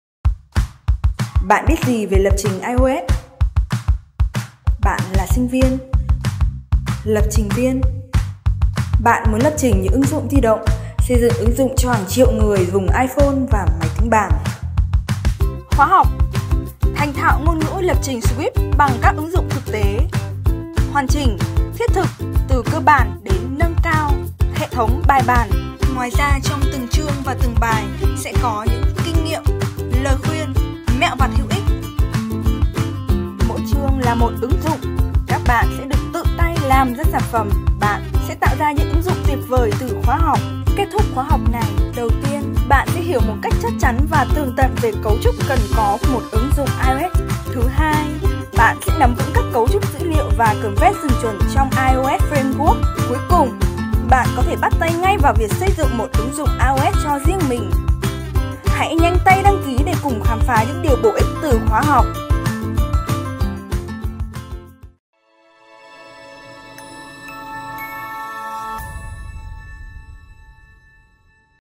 Thành thạo ngôn ngữ lập trình Swift bằng các ứng dụng thực tế
Lập trình iOS Swift đang rất hot hiện nay và đang được nhiều nhà tuyển dụng tìm kiếm.

Khóa học “Thành thạo ngôn ngữ lập trình Swift bằng các ứng dụng thực tế” trang bị các kiến thức, kỹ năng để bạn trở thành Lập trình viên chuyên nghiệp trên nền tảng các thiết bị của Apple như iPhone, iPad, iWatch, Macbook… Từ đó, giúp bạn sẵn sàng tiếp nhận công việc tại các công ty phần mềm hiện nay.

Khóa học gồm 11 chương

Chương 1 là giới thiệu qua về môi trường làm việc (XCode 7, iOS 9, Swift 2)

Từ chương 2 đến 11 là 10 ứng dụng hoàn chỉnh do chính Giảng viên làm ra, sẽ được sử dụng làm ví dụ thực tế để dẫn dắt các bạn tiếp thu các kiến thức từ đơn giản đến phức tạp của iOS 9

Các bạn có thể học tuần tự từng chương một, hoặc chọn 1 chương bất kỳ để bắt đầu tùy thuộc vào mức độ thành thạo của bạn đối với iOS. Không nhất thiết phải bắt đầu từ chương 1 nếu bạn đã biết qua 1 chút ít, nhưng hãy xem qua các bài đầu tiên để hiểu rõ hơn cách thức hoạt động của 1 ứng dụng iOS hoàn chỉnh!

Cách tiếp cận đi từ thực tế: thông qua 10 ứng dụng iOS với độ phức tạp từ thấp đến cao, khóa học sẽ mang lại cho các bạn 1 trải nghiệm học tập tự nhiên nhất, thay vì phải tự tay mò mẫm những kiến thức rời rạc trong hàng tháng trời, các bạn sẽ chỉ mất 2 tuần để nắm được các kiến thức cơ bản nhất của việc lập trình 1 ứng dụng iOS, từ đó nâng cao khả năng tự học & tự định hướng nâng cao chuyên môn về iOS của mình.

Mỗi chương là một ứng dụng: các bạn sẽ được tự tay làm ra sản phẩm ngay mà không phải học hết toàn bộ khóa học. Tại mỗi chương, giảng viên sẽ giới thiệu với các bạn các bước tuần tự để làm ra ứng dụng hoàn chỉnh, kèm theo những kiến thức cơ bản về hệ thống & framework. Tất nhiên các bạn cũng sẽ được truy cập tới đầy đủ mã nguồn của các sản phẩm được sử dụng trong khóa học.

Bạn sẽ học được gì?
Hiểu một cách chắc chắn và tường tận về cấu trúc cần có của một ứng dụng iOS 9 dùng Swift 2
Dễ dàng tiếp nhận các công việc liên quan tại những công ty phần mềm
Nắm vững các cấu trúc dữ liệu & convention chuẩn trong iOS framework
Có thể bắt tay ngay vào việc xây dựng 1 ứng dụng iOS cho riêng mình
Đối tượng đào tạo
Các bạn sinh viên mới / sắp tốt nghiệp ra trường, muốn trang bị các kiến thức về lập trình iOS nói riêng và lập trình ứng dụng di động nói chung để tìm kiếm một công việc phù hợp với mức lương cạnh tranh
Các lập trình viên đã có kinh nghiệm với những nền tảng không phải di động muốn học thêm hoặc chuyển sang lĩnh vực lập trình di động
Các lập trình viên đã có kinh nghiệm với Android muốn học nhanh iOS để bổ sung thêm các kỹ năng chuyên môn phục vụ nhu cầu phát triển công việc